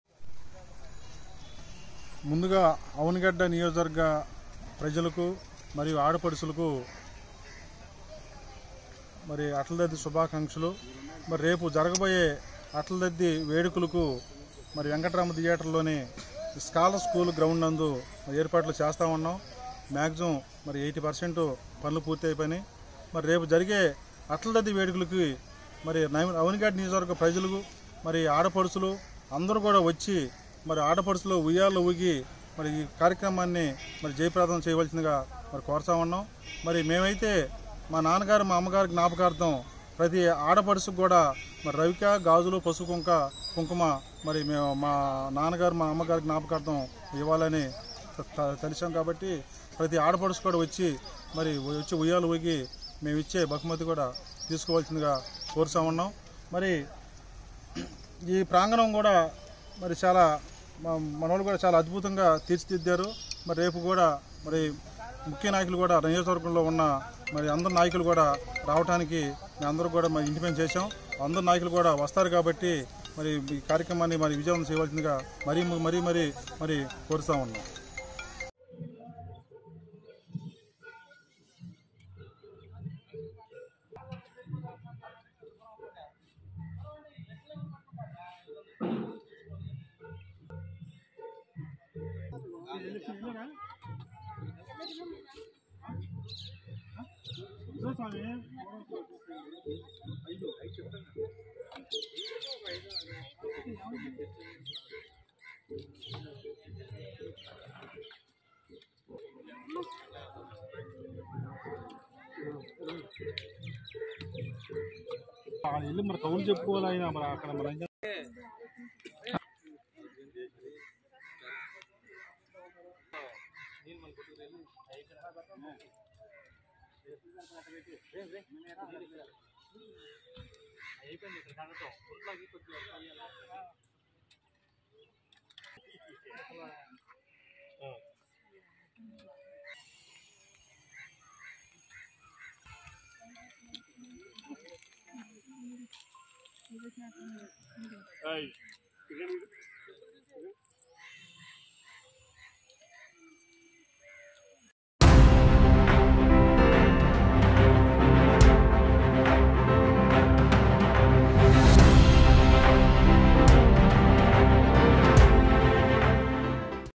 నాగాయలంకలో చిలకలపూడి పాపారావు సారథ్యంలో అట్లతద్ది వేడుకలు...
స్పాట్ న్యూస్ దివిసీమ JRS